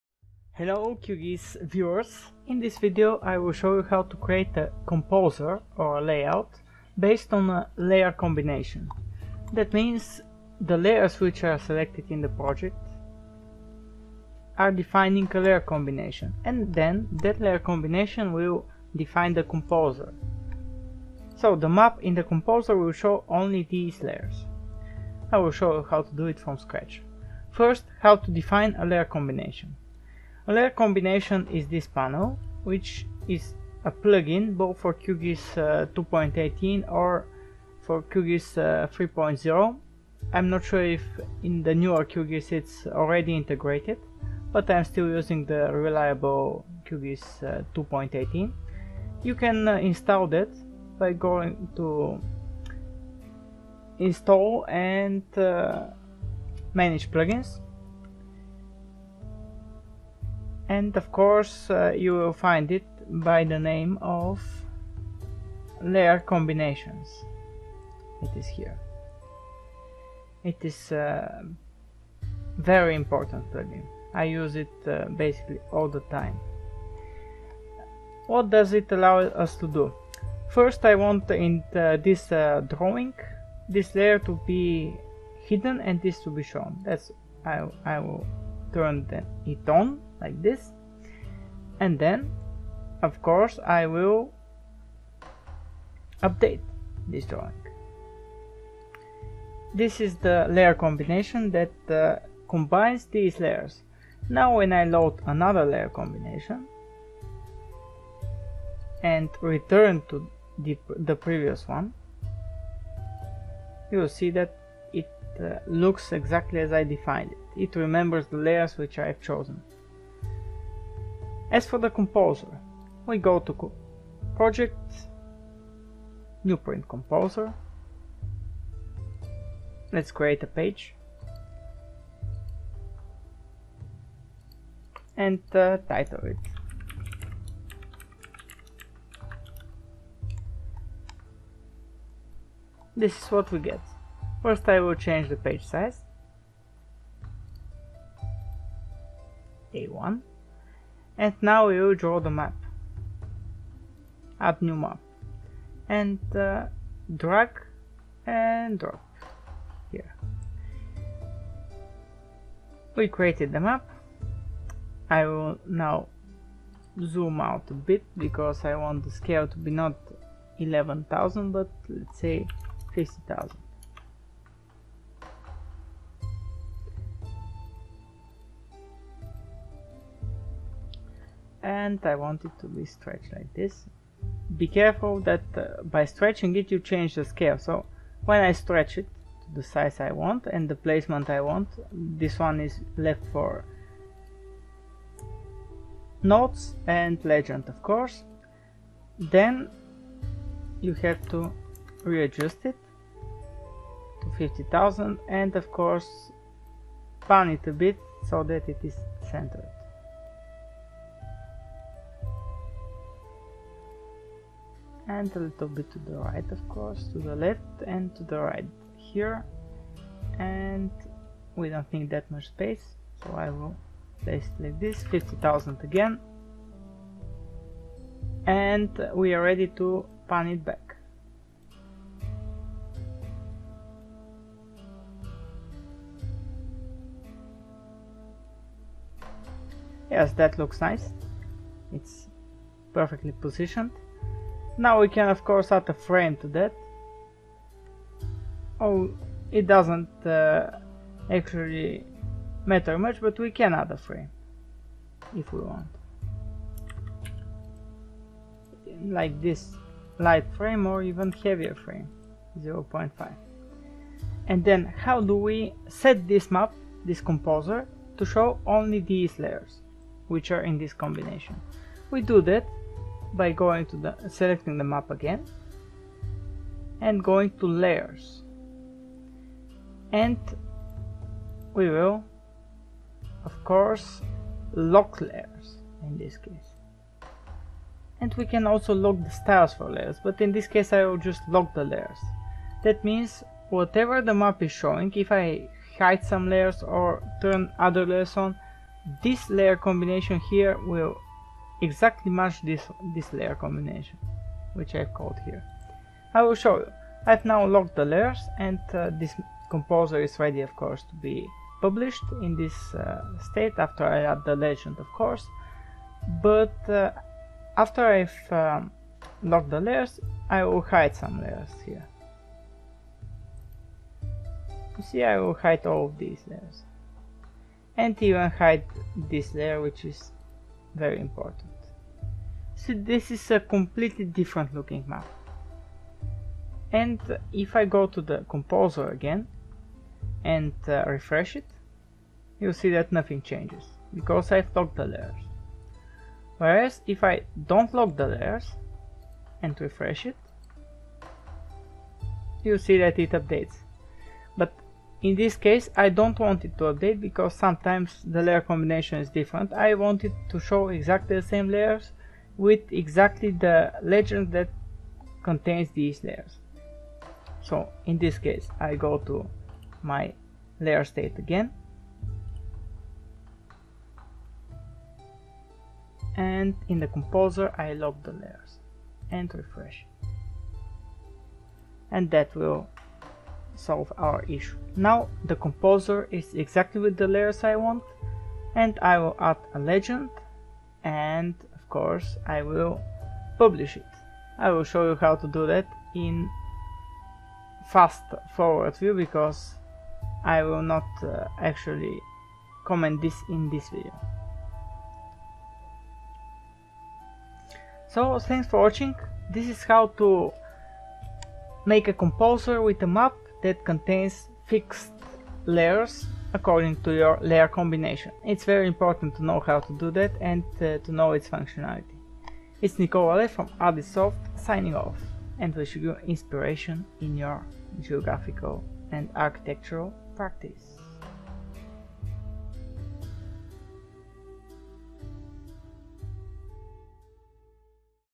Lock composer layers to match layer combination in QGIS
Want to always make perfect maps that match the layers you want? This is one great way to set it and never make mistakes with the wrong layers showing on the qgis composer. You can first set the layer combination by using the plugin with the same name. Then, in the print composer, you lock the layers to match that layer combination. Finally, you can create a legend that contains the layers and never change it again.

Author/Автор: Nicola Lalev from ADIsoft
Date/Дата: 01/07/2019
Programs used / Използван софтуер:

-QGIS 2.18